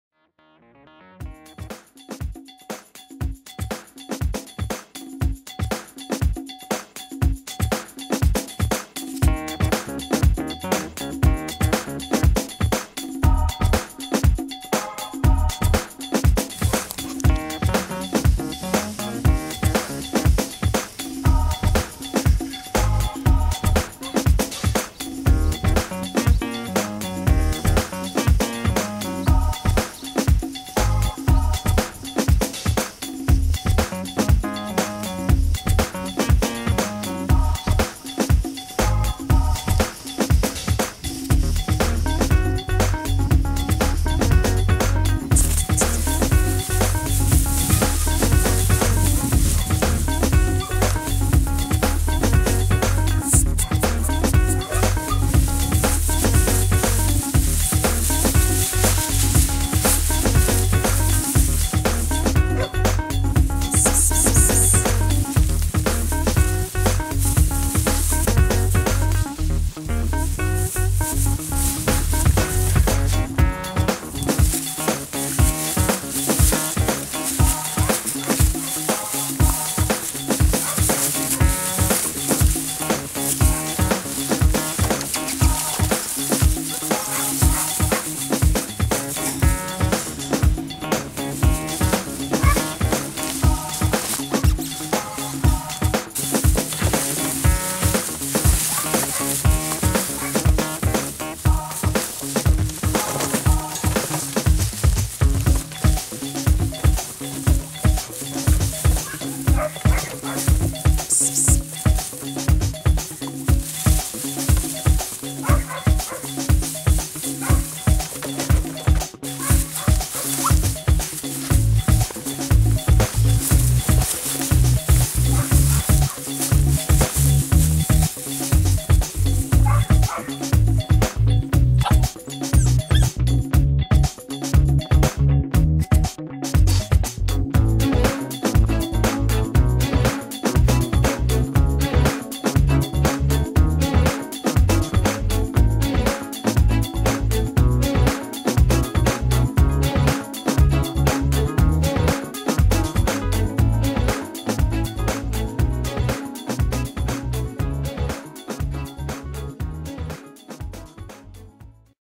Kelpie puppy, Female exposed to goats at 11 weeks.
Born 8.3.2015; and is 11 weeks old in this video.  Double bred Urricelqui Ranch Kelpies.  Out of Urricelqui April (Hall J.C. X Urricelqui Bullet), and by Urricelqui Mitchell (Billie) (Hall J.C. X Jumakola Striker).